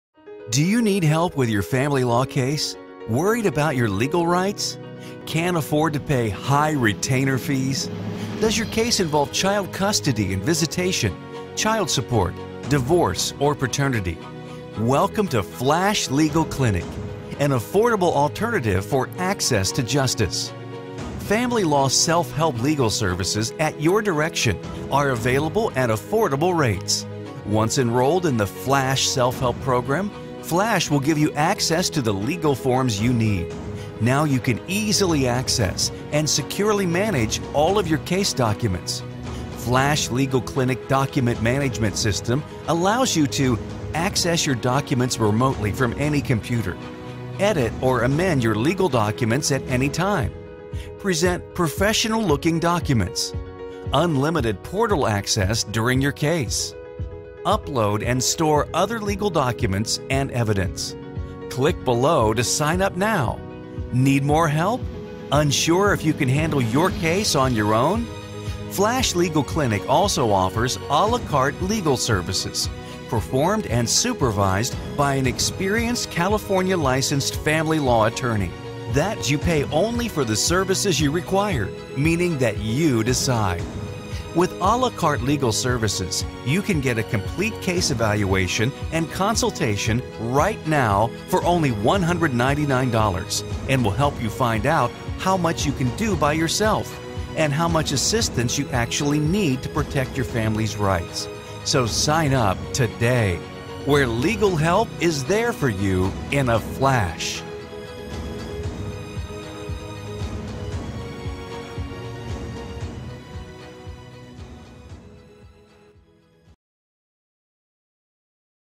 Flash Legal Clinic | Self Legal Help Services In California
Do you NEED help with your family law case! Worried about your legal rights!

Can't afford to pay HIGH RETAINER FEES?
Does your case involve - Child Custody and Visitation - Child Support – Divorce or Paternity?

Welcome to FLASH LEGAL CLINIC! An affordable alternative for ACCESS to JUSTICE.

FAMILY LAW "SELF-HELP" legal services, at your direction, are available at affordable rates.

Once enrolled in the FLASH Self-Help program, FLASH will give you access to the legal forms you need.

Now you can easily access and securely manage all of your case documents.

FLASH LEGAL CLINIC document management system allows you to:
• Access your documents remotely from any computer
• Edit or Amend your legal documents at any time
• Present professional looking documents
• Unlimited “portal” access during your case
• Upload and store other legal documents and evidence

Need more help? Unsure if you can handle your case on your own?
FLASH Legal Clinic also offers…“Ah LAW CARTE” legal services performed and supervised by an experienced California licensed Family Law Attorney.

That means you pay only for the services you require…. Meaning that “YOU” decide!

With AH LAW CARTE legal services, you can get a complete case evaluation and consultation RIGHT NOW for only $199.00 and will help you find out ….

How much you can do by yourself

And How much assistance you actually need to protect your family’s rights

“Where legal help is there for you in a FLASH!”